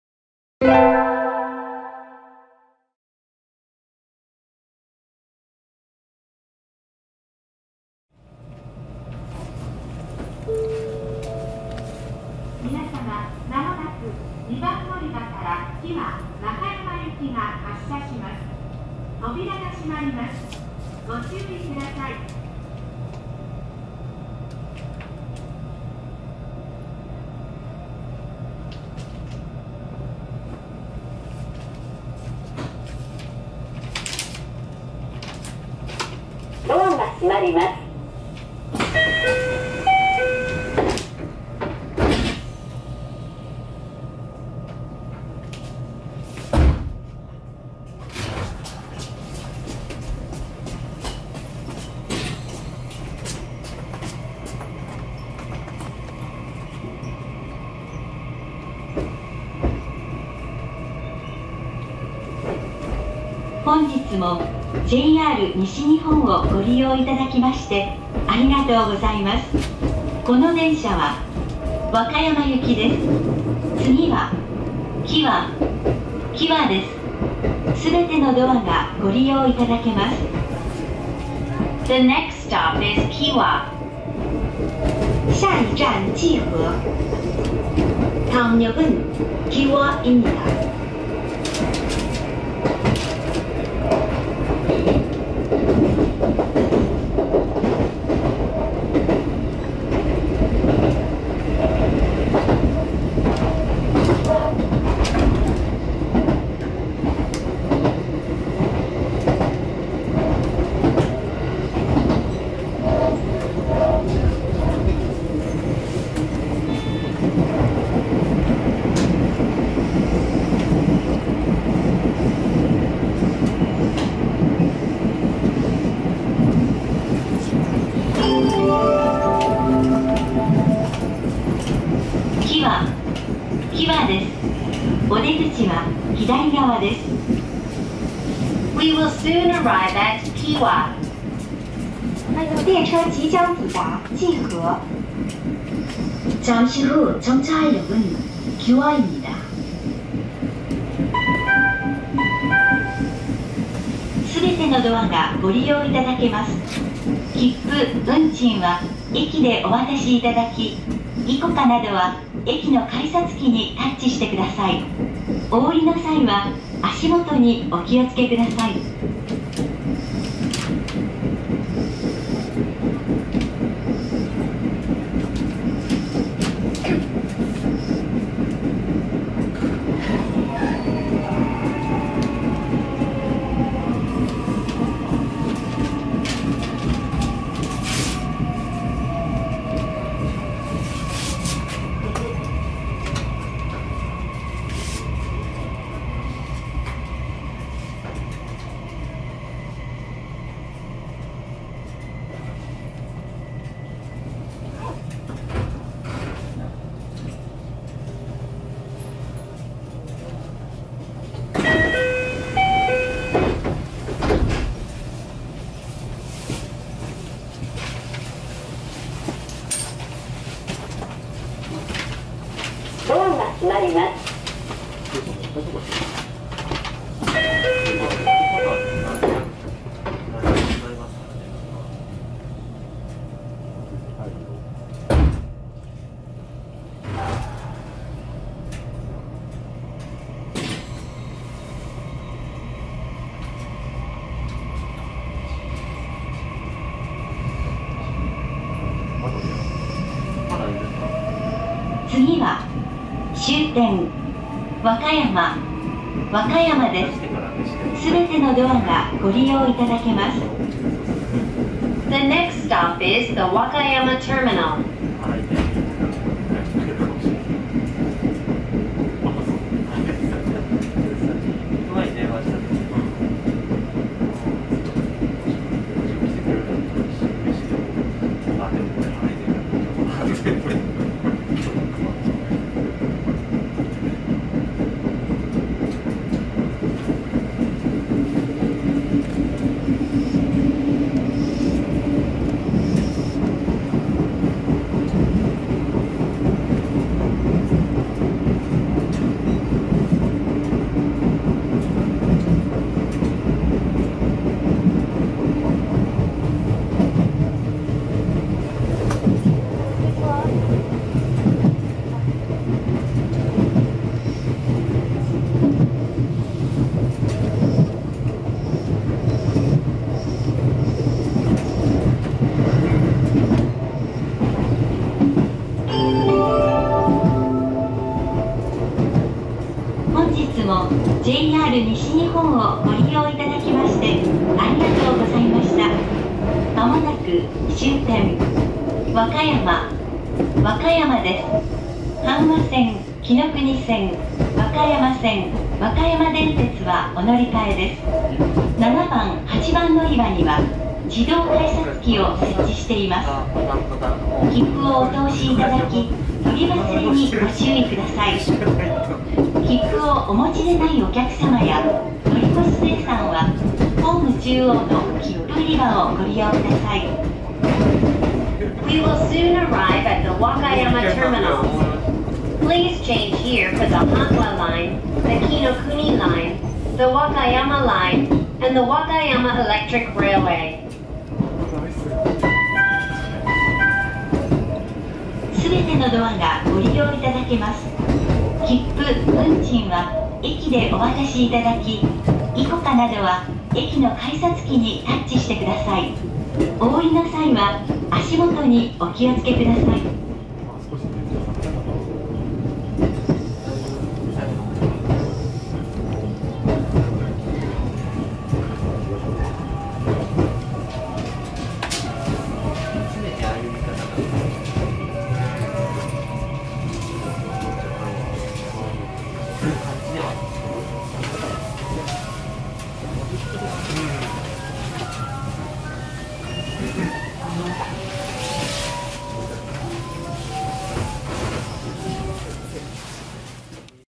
【4K前面展望】紀勢本線(和歌山市～和歌山）
〇2022/12〇曇〇
経由; 西日本旅客鉄道紀勢本線
種別; 普通
運行/収録; 和歌山市ー和歌山
車種; 227系1000番台
Via; West Japan Railway Kisei Line
Type; Local
Run./Rec.; Wakayamashi～Wakayama
Model; 227-1000 Series

《停車駅; 着-発 Station; Arr.-Dep.》
紀和 Kiwa; 3:25-3:59

《乗換案内 Transfer》
▼和歌山 Wakayama
〇きのくに線 Kinokuni Line

〇阪和線 Hanwa Line

〇和歌山線 Wakayama Line

◎和歌山電鐵 Wakayama Electric Railway